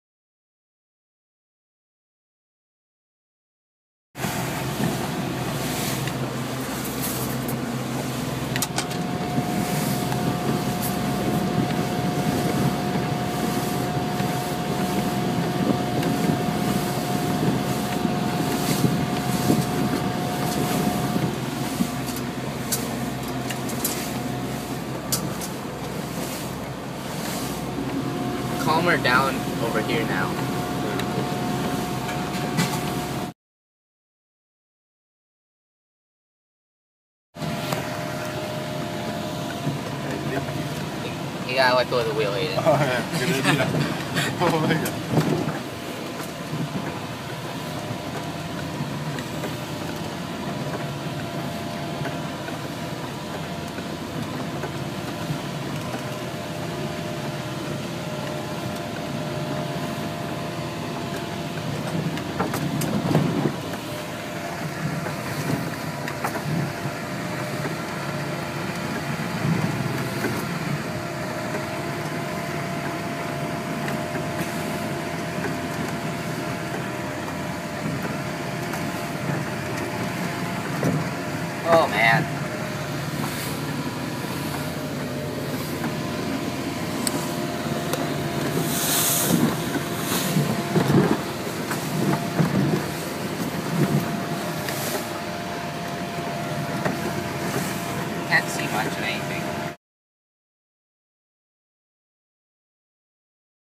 26' Tollycraft Big Waves
In the Strait of Georgia on our way back from our may long weekend at Plumper Cove, Keats Island. Blowing about 20-25 knots, we were just rounding Cape Roger Curtis heading towards the North Arm of the Fraser River. About half way to the river there was a couple of very big waves where the entire bow of the boat went completely under water and that happened more than once. We were travelling about 7-7.5 knots burning 6GPH.